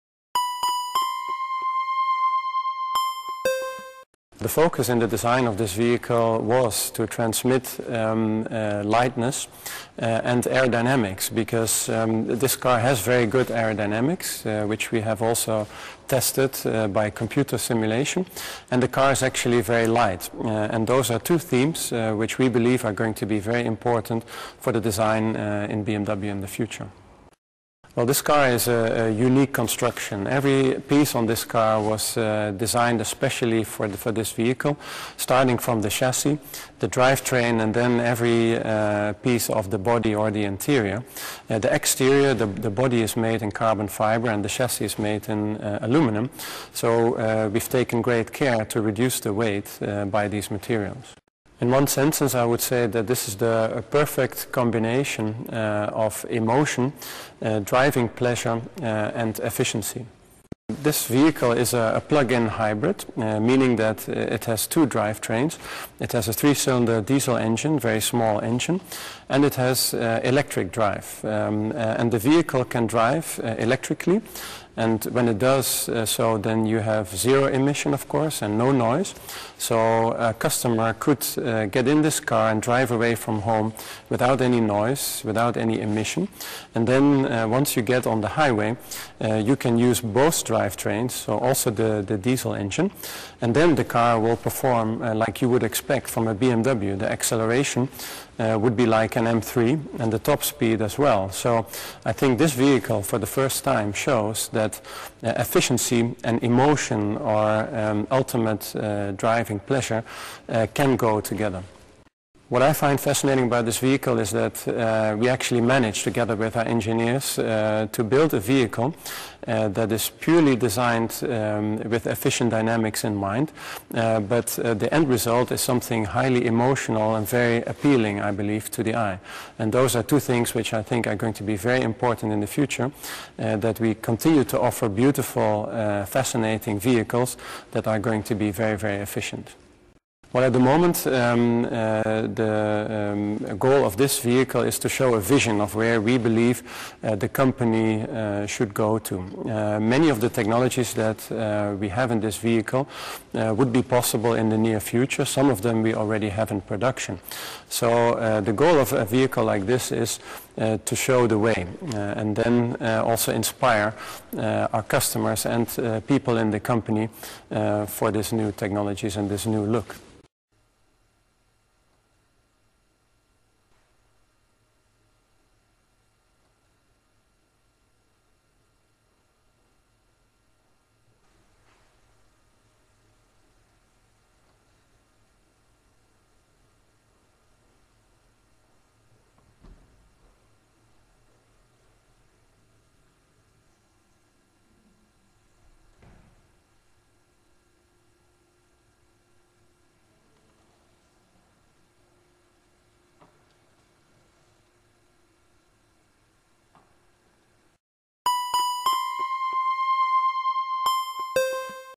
Adrian van Hooydonk, Head of Design, BMW Group
Adrian van Hooydonk, Head of Design, BMW Group, comments on BMW Vision EfficientDynamics.